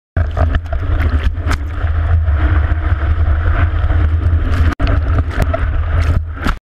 Lola Lolita Loca Teeny Waves on my Hull Shape
PROOF you can Catch Teeny Tiny Mushy Surf on a Planing Hull. This Shape is PERFECT for Girls!! This is my Lolita Loca Twinzer Hull Shape.  I Love this Fun Board!!